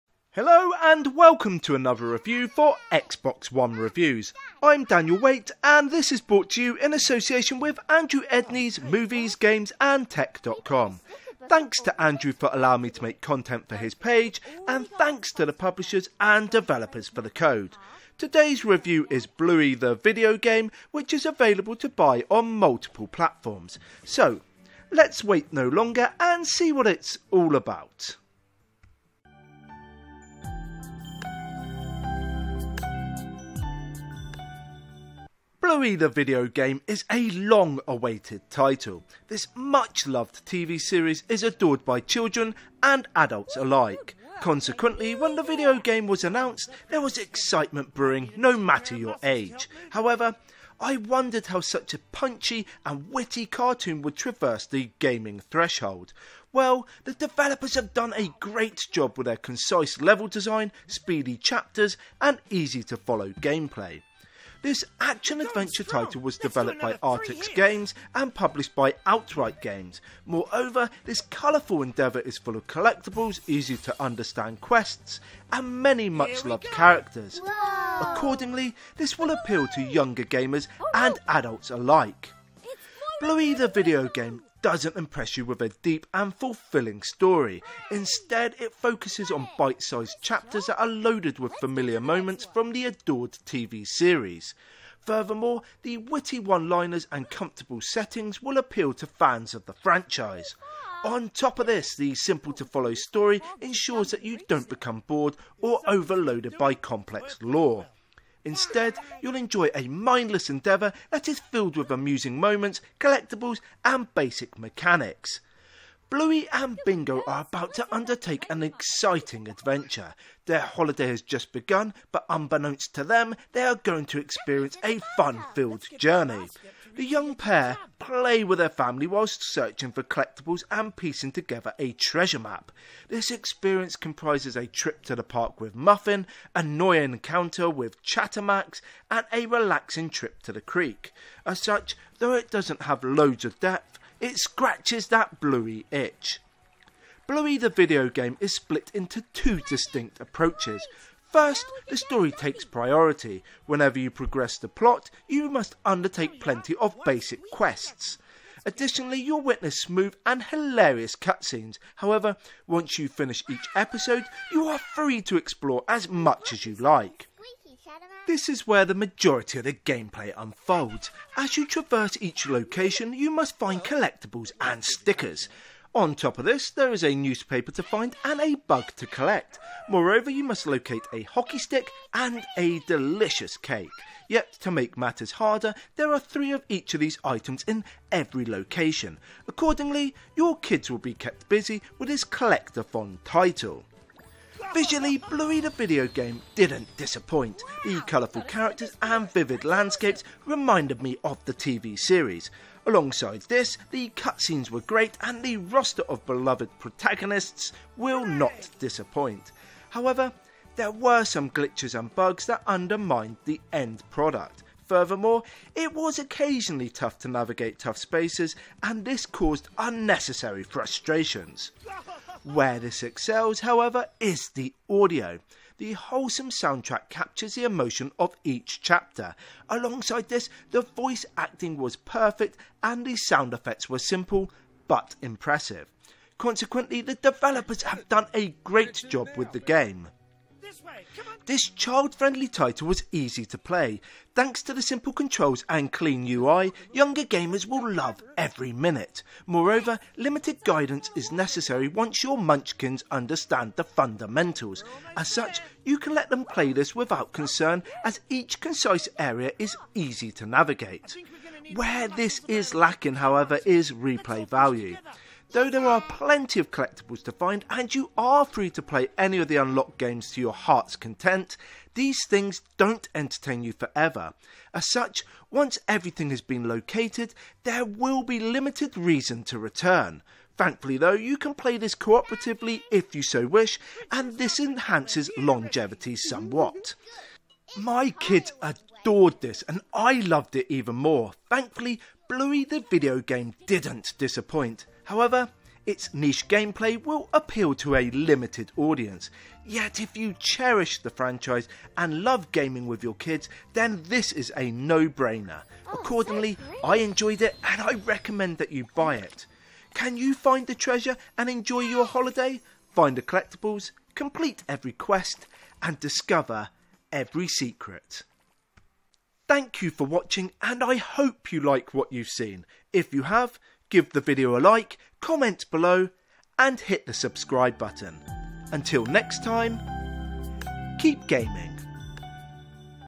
Bluey: The Videogame review
Bluey: The Videogame is a fantastic child-friendly endeavour. What's more, it'll appeal to fans of the franchise. Accordingly, if you are looking for a challenging game, this isn't for you. However, if you want a straightforward, fun, and colourful affair, then this ticks every box.

Available on the Xbox Series X/S, Xbox One, PC, Nintendo Switch, PlayStation 4 and PlayStation 5.

Thanks to Artax Games and Outright Games for the code.